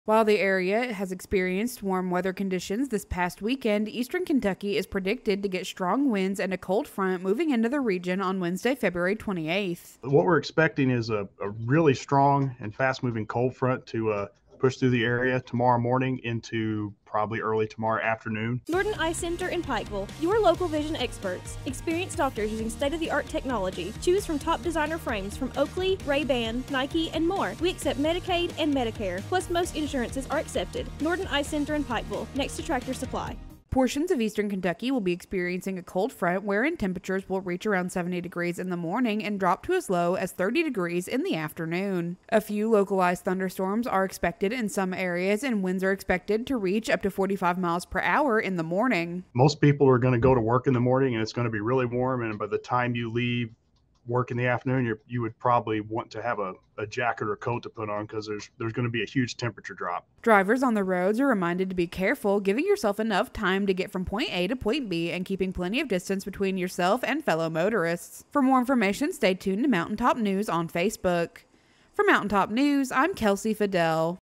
A Large Temperature Change and Strong Winds are Expected for 02/28/24
The temperature high for February 28, 2024, is expected to reach the 70s, but it should fall into the 30s during the afternoon hours.  Strong winds are also expected.